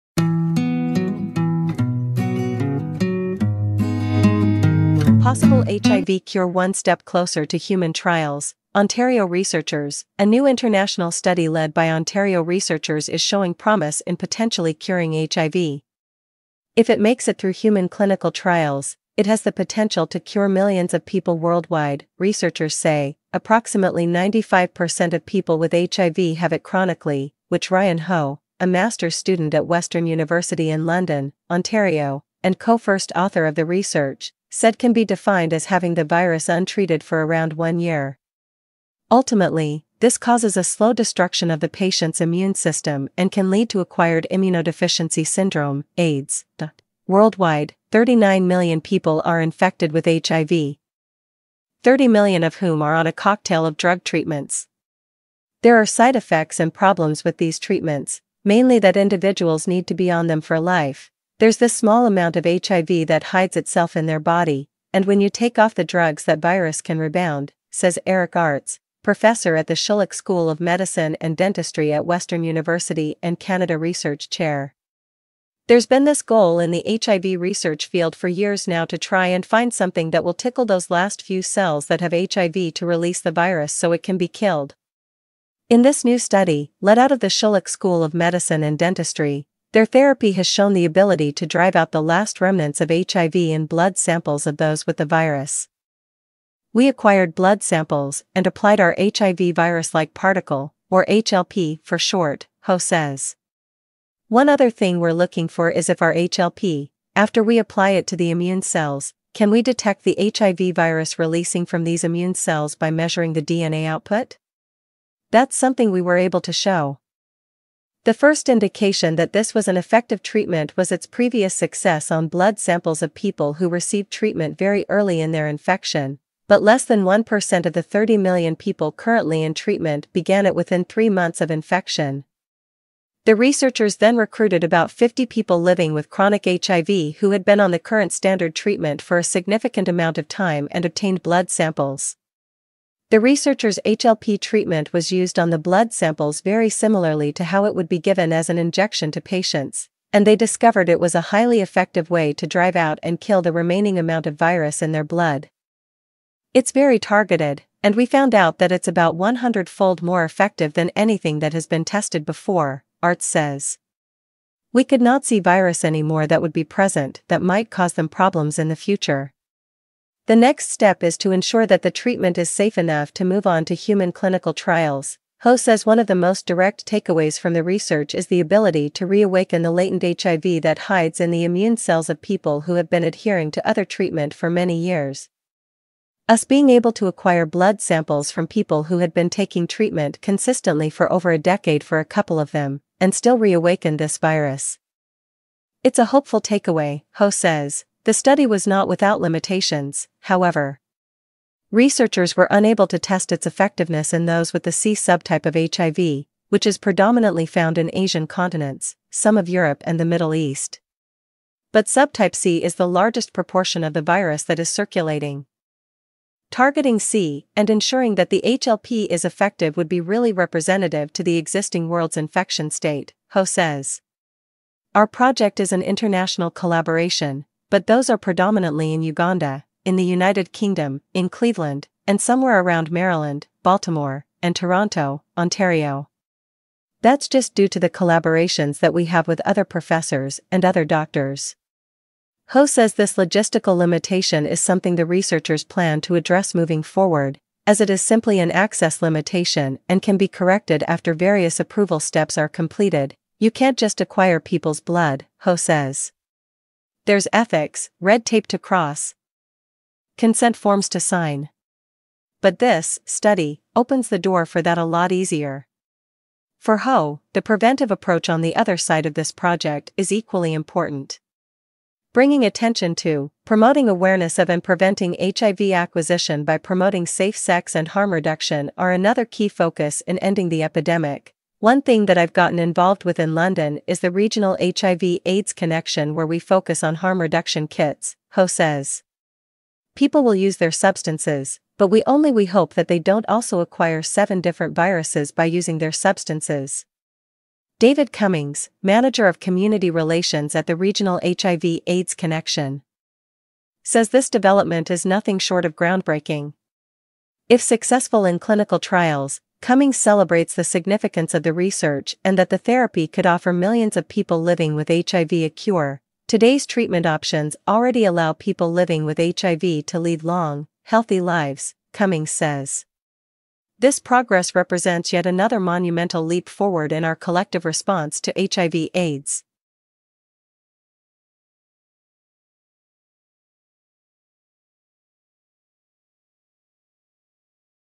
Possible HIV cure 1 step closer to human trials: Ontario researchers
A new international study led by Ontario researchers is showing promise in potentially curing HIV. If it makes it through human clinical trials, it has the potential to cure millions of people worldwide, researchers say.
Approximately 95 per cent of people with HIV have it chronically, which Ryan Ho, a master’s student at Western University in London, Ont., and co-first author of the research, said can be defined as having the virus untreated for around one year. Ultimately, this causes a slow destruction of the patient’s immune system and can lead to acquired immunodeficiency syndrome (AIDS).
Worldwide, 39 million people are infected with HIV, 30 million of whom are on a cocktail of drug treatments. There are side effects and problems with these treatments, mainly that individuals need to be on them for life.